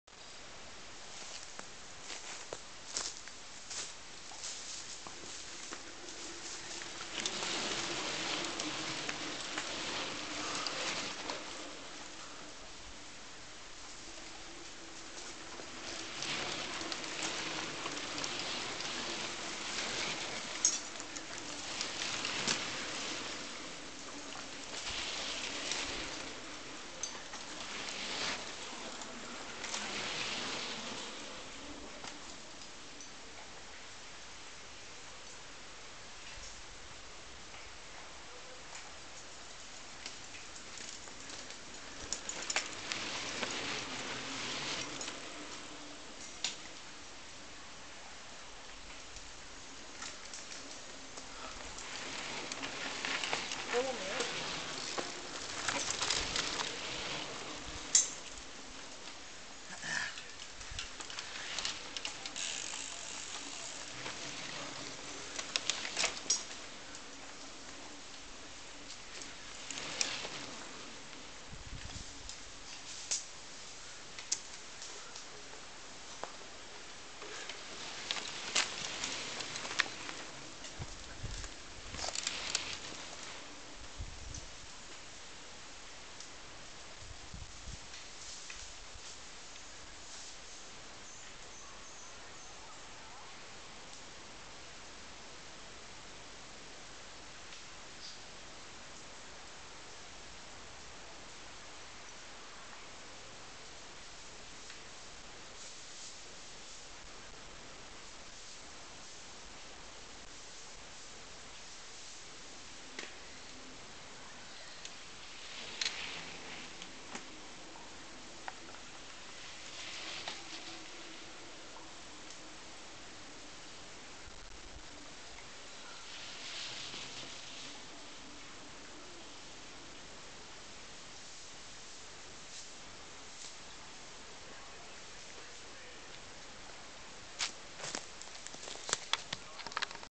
ATB KEMPEN CLIP DB RIT6 - CD klasse
Eerste bos doorkomst van de CD klasse in oefenrit 6 van ATB Kempen